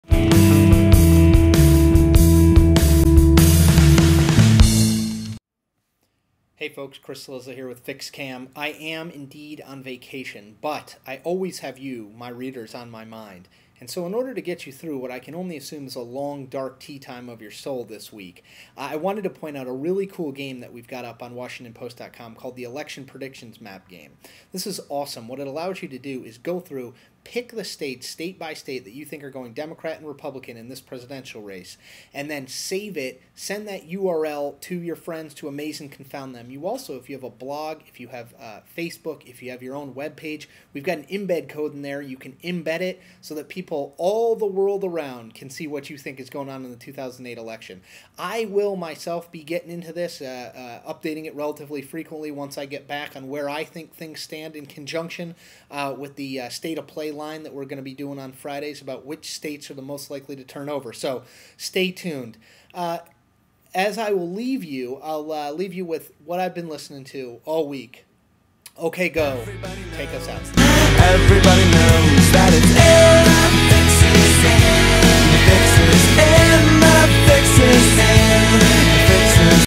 FixCam: Introducing the Electoral Map
The newest feature from washingtonpost.com. Choose how you think the general election will play out -- state by state.